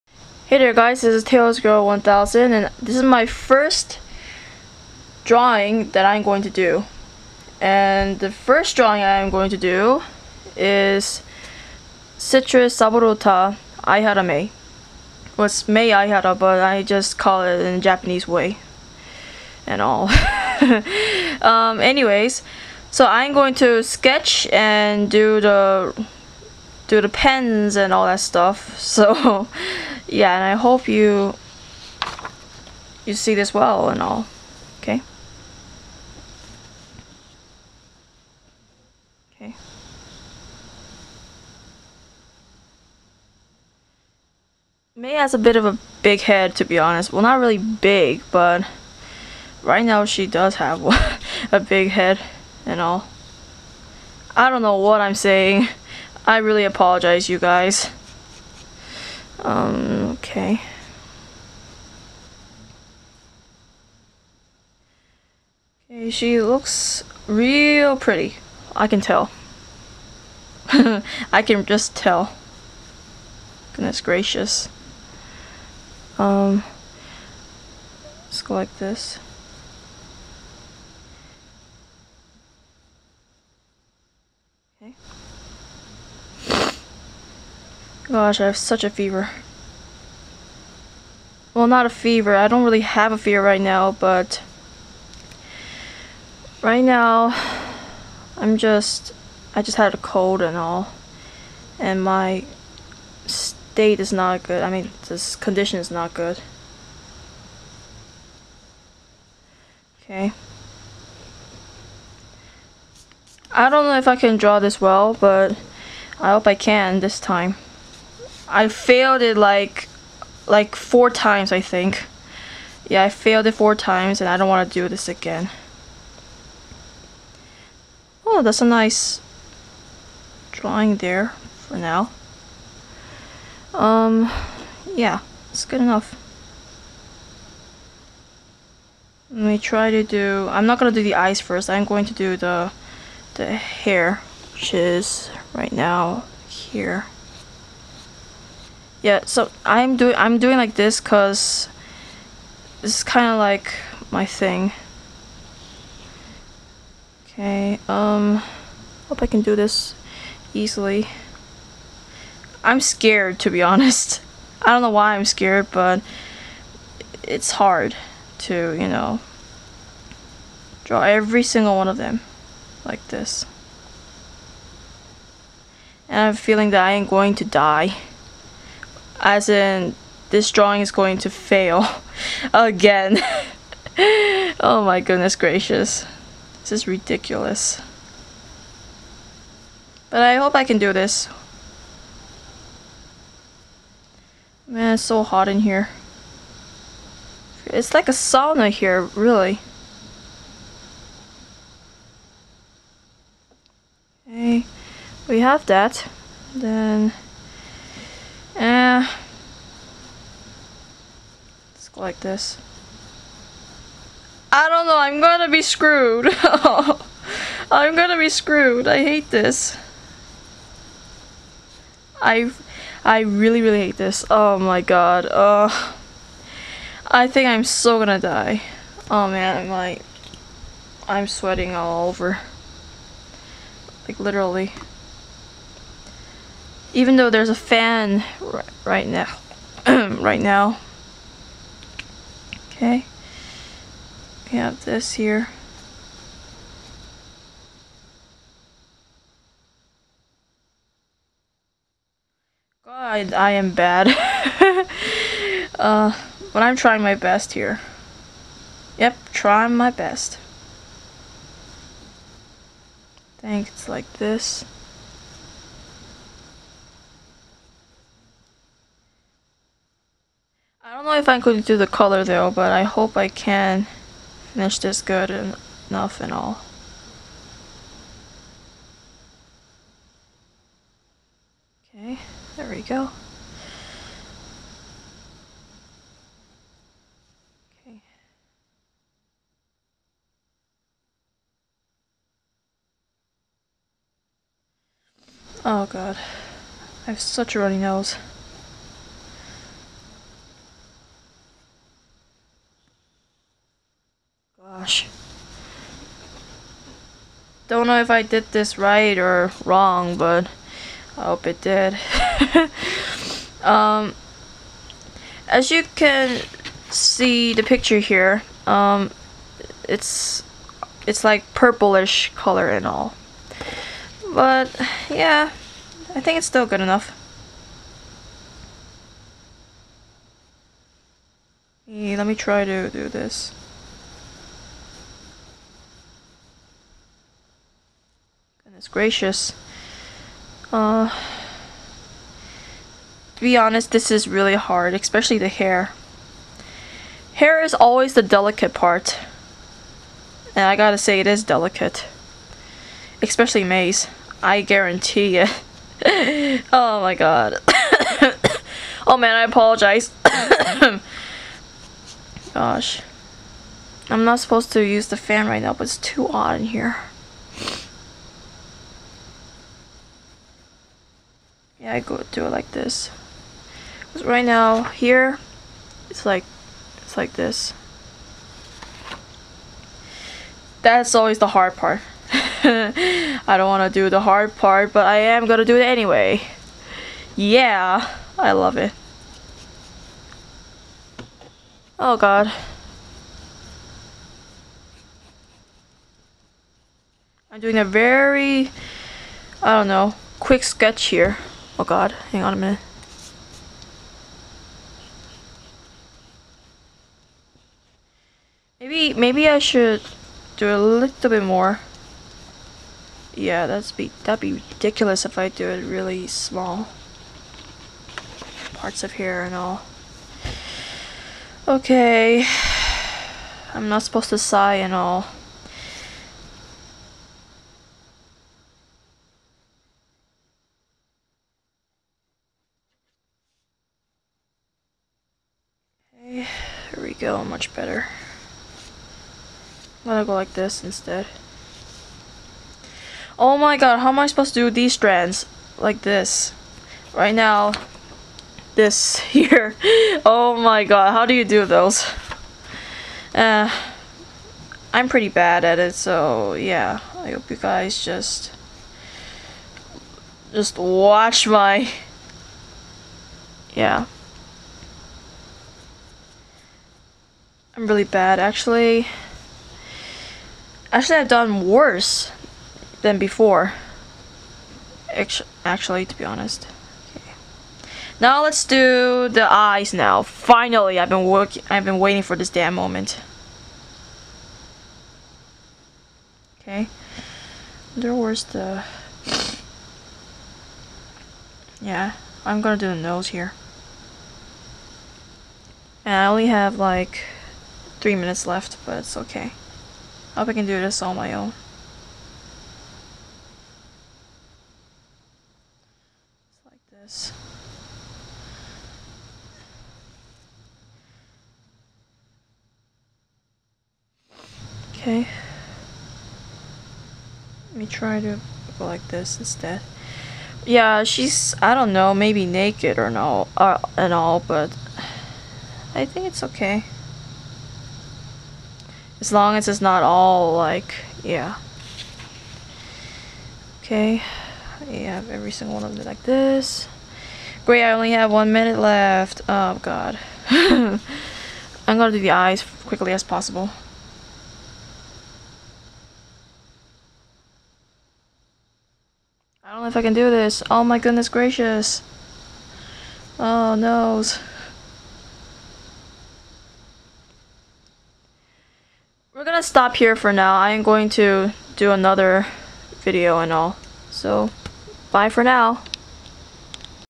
Citrus Saburo Uta Mei drawing sketch part 1
Hi there it's TalesGirl1000
and this is my first time doing this
video and I drew Aihara Mei LOL
Well.. First time doing the drawing with
voice in it haha.
I hope you enjoy it~~ LOL ^^